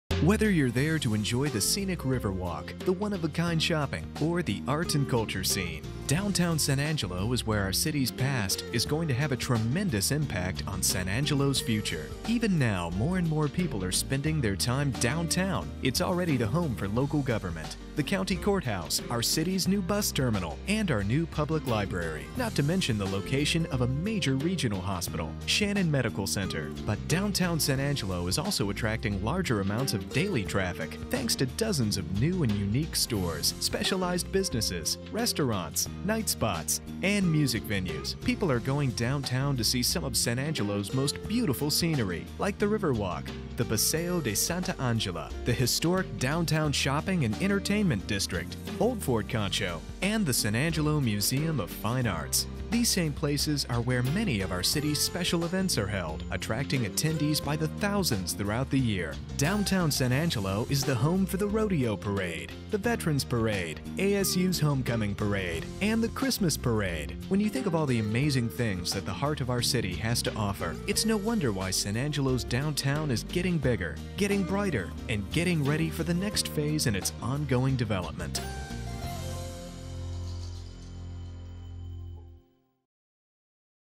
Downtown San Angelo - Come experience Downtown San Angelo
Welcome to Historic Downtown San Angelo, where a mix of modern art and a Western heritage create a socially inclusive experience. Explore, play, dine, celebrate, shop, and relax with your family and friends. The words used to describe Historic Downtown San Angelo are as numerous and diverse as the people who live and visit here.

Video created by McLaughlin Advertising 2014
Downtown San Angelo